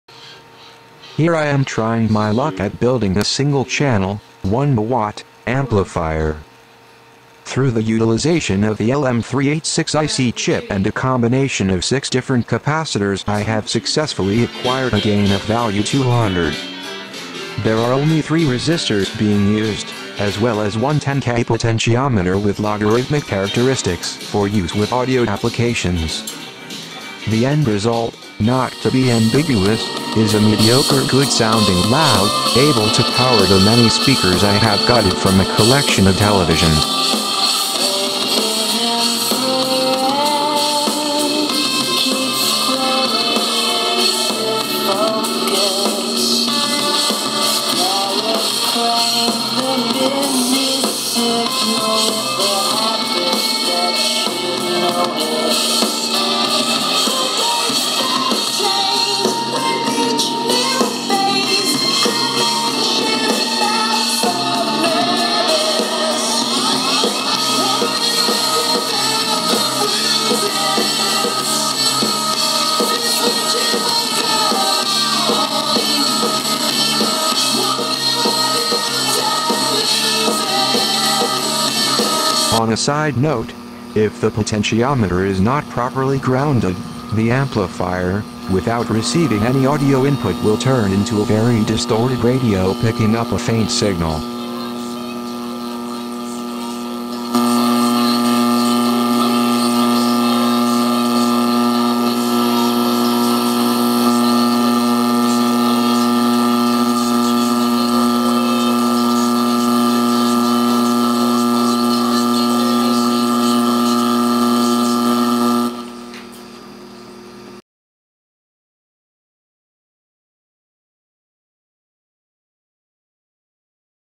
LM386 Breadboard
Inspired by the thought of putting an amplifier into a Altoids can from the CMoy headphone amp designs out in the world.

I needed something that could actually power speakers at a low voltage as well as being small enough that it too could join in on the Altoids can fun.

End result is not at all close to being bad, which by default makes it good.

1 watt of power (theoretically), and a gain of 200